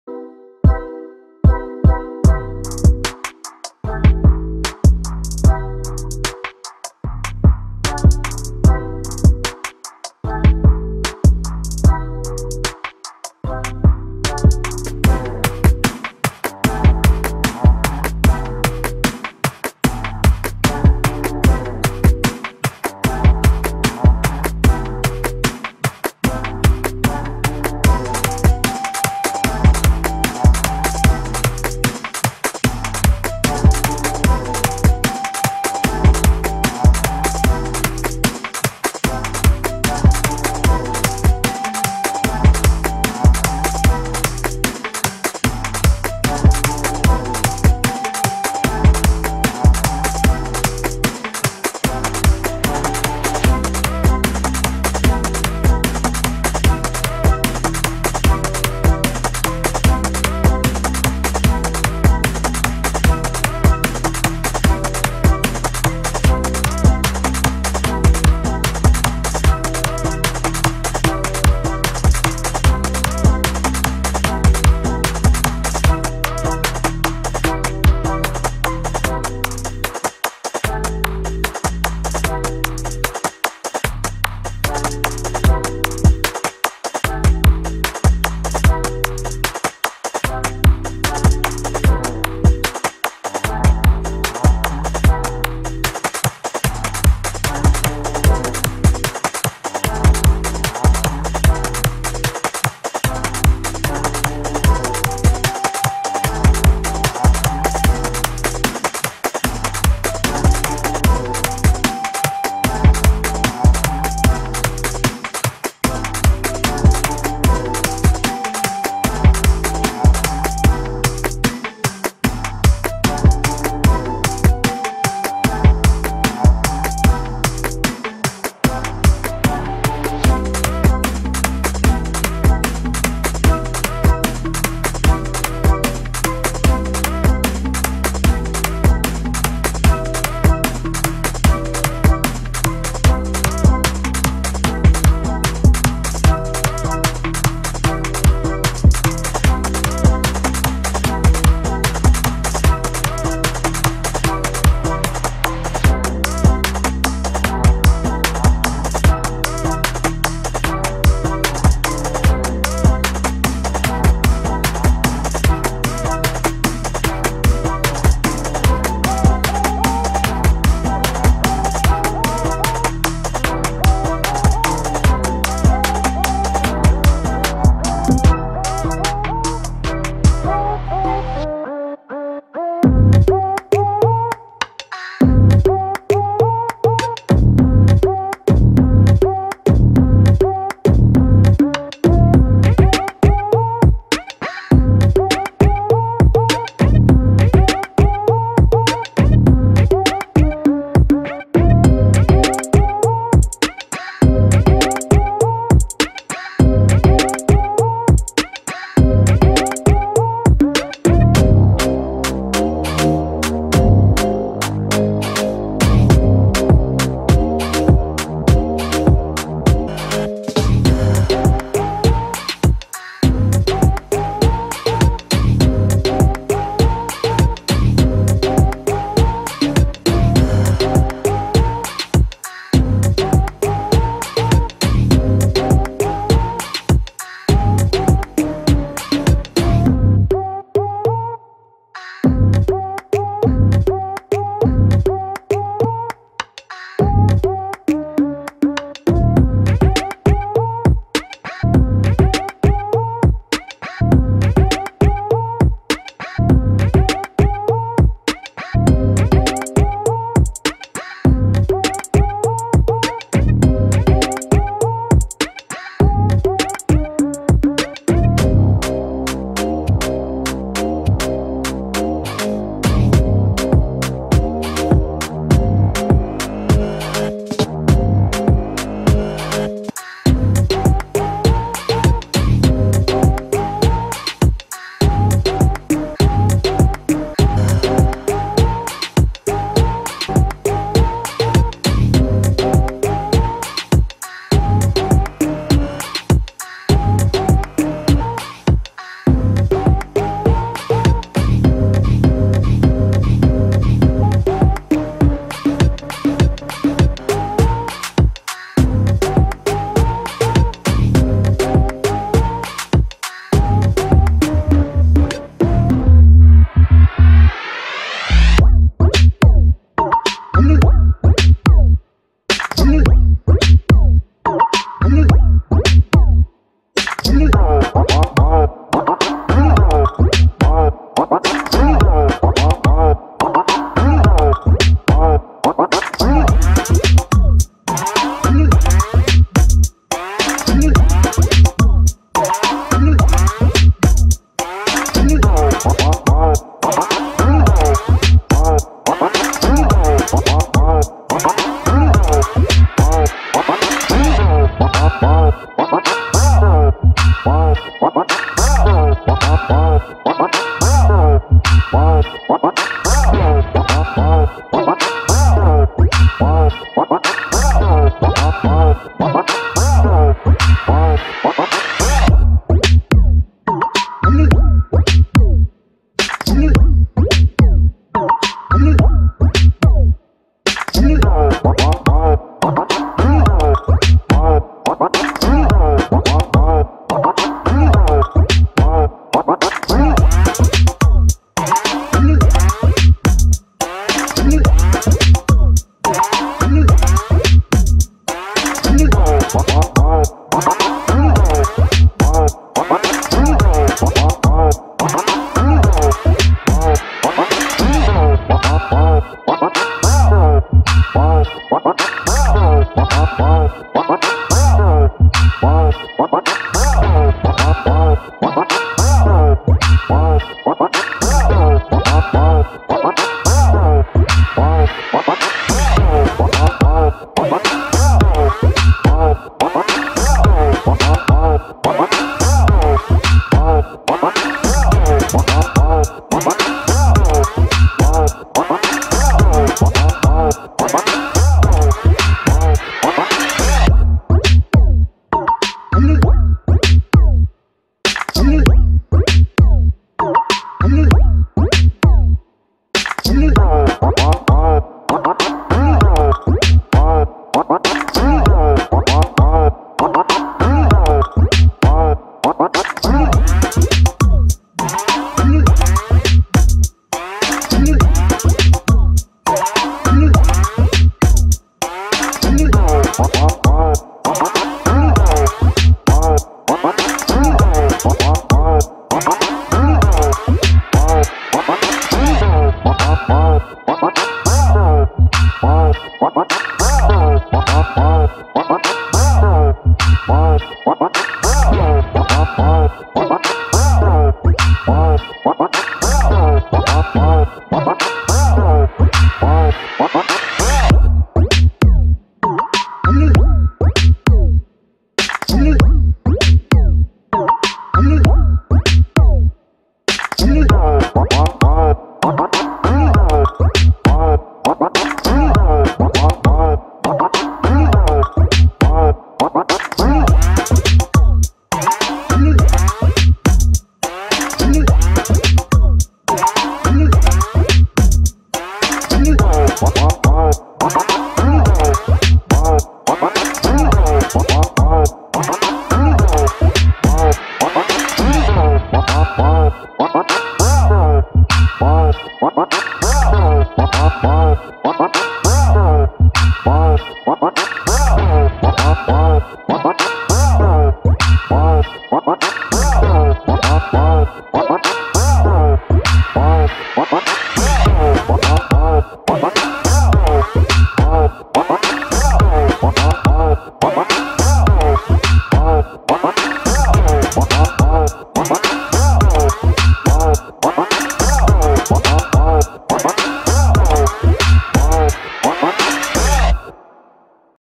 COOL MIX 2020 dj mano
hoi deze mix duurt / 9min : 37sec / gemixt met you tube muziek/audio bibliotheek... veel luister plezier.